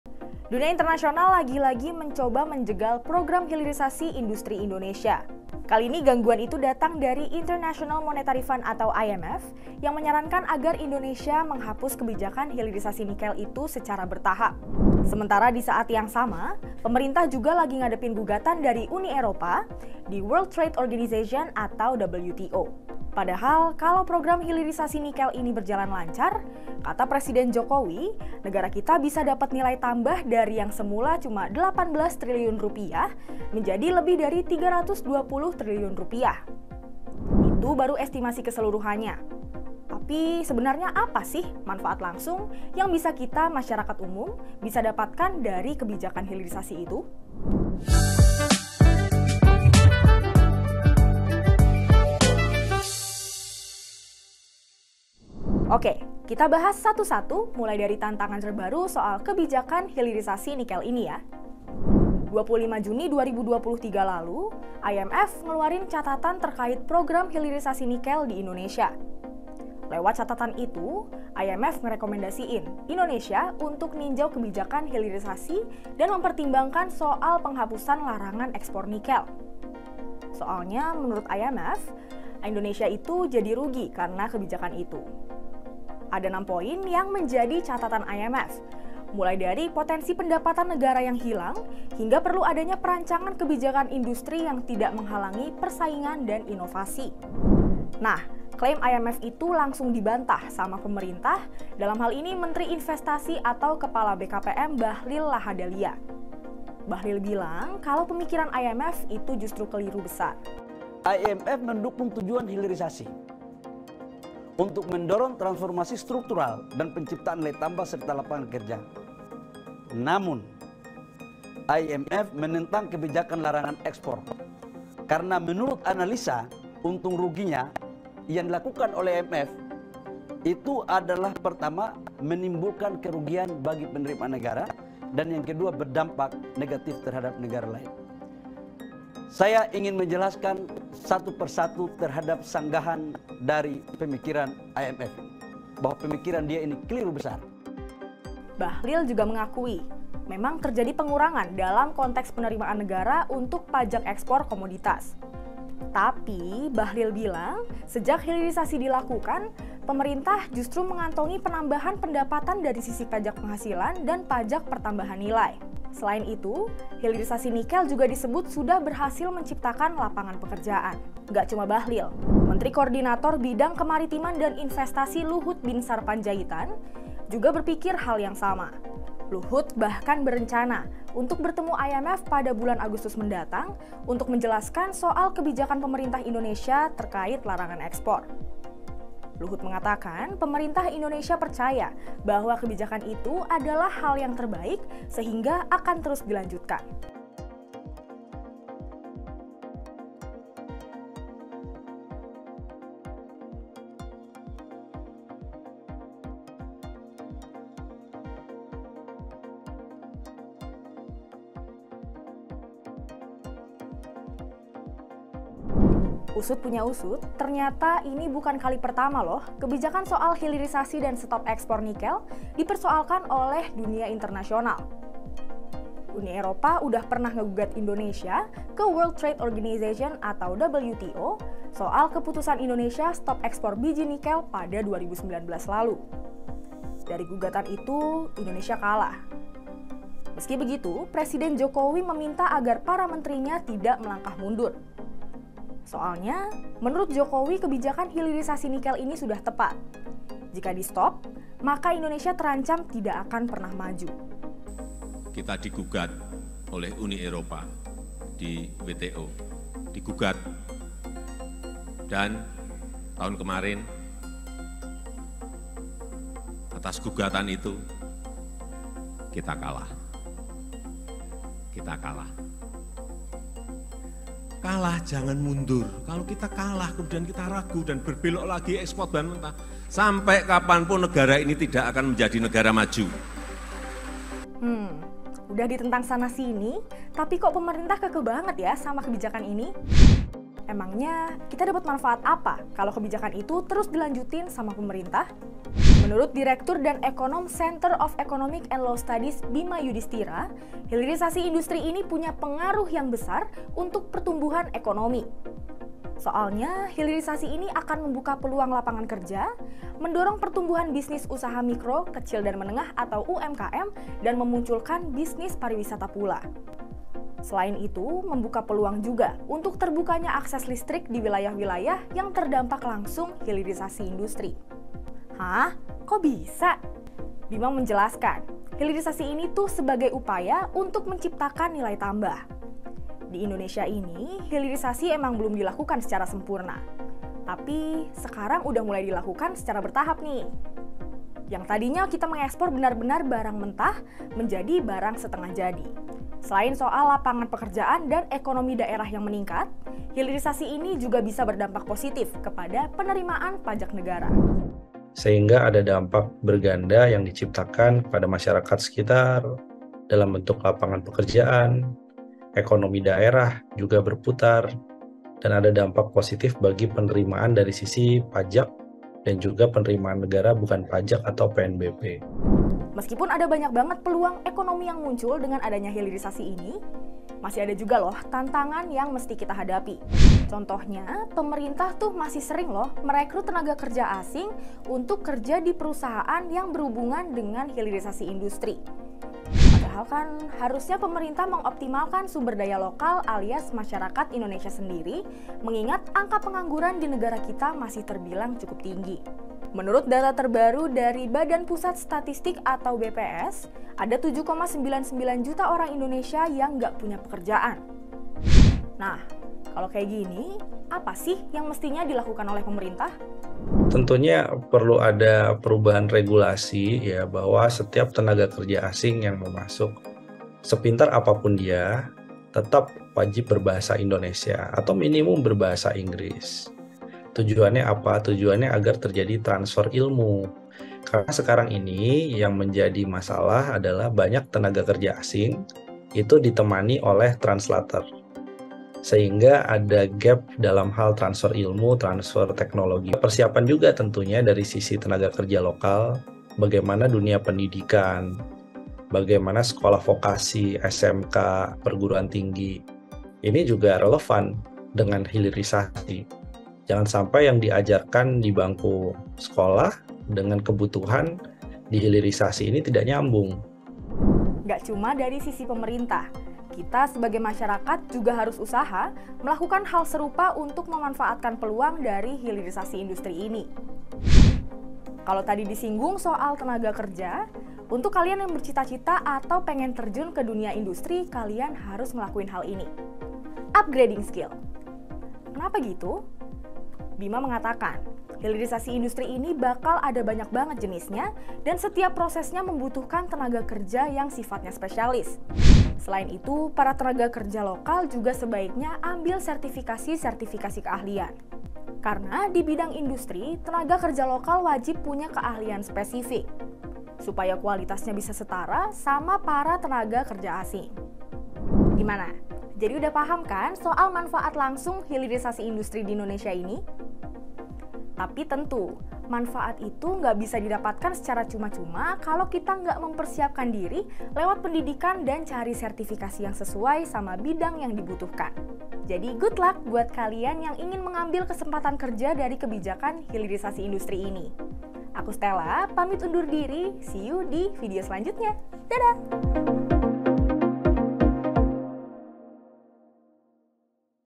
[GENERASI CUAN] Hilirisasi Industri, Apa Dampak dan Keuntungannya bagi Kita?
Lembaga Dana Moneter Internasional atau IMF mengeluarkan catatan terkait program hilirisasi nikel di Indonesia pada 25 Juni 2023.

Dalam catatannya itu, IMF merekomendasikan Indonesia untuk meninjau kebijakan hilirisasi dan mempertimbangkan soal penghapusan larangan ekspor nikel.

Sebab, menurut IMF kebijakan hilirisasi bahan mentah tersebut justru membuat Indonesia merugi.

Namun, terlepas dari kontroversi soal implementasi kebijakan hilirisasi industri dan larangan ekspor nikel ini, sebenarnya apa manfaat dan dampak yang bisa langsung dirasakan oleh masyarakat Indonesia?

Penulis Naskah: Chrisstella Efivania Rosaline
Host: Chrisstella Efivania Rosaline
Kreatif: Chrisstella Efivania Rosaline
 Videografer: Timothy Afryano
Video Editor: Lulu C Mahendra
Produser: Adesari Aviningtyas